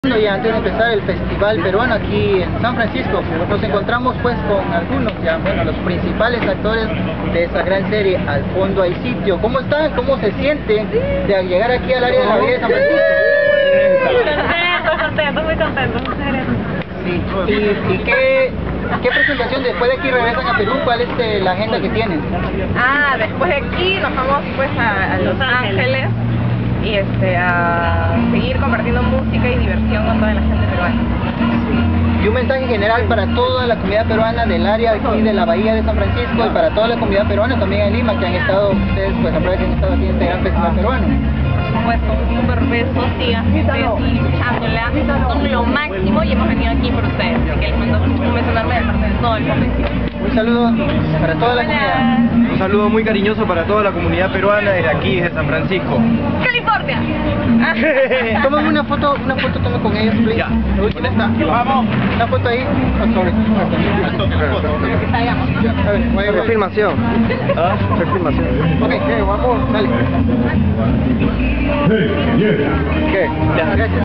Al Fondo Hay Sitio. 4to Festival Peruano de San Francisco.
Visita de parte del elenco de la serie peruana Al Fondo Hay Sitio, que se transmite diariamente en Peru y se puede ver tambien en USA.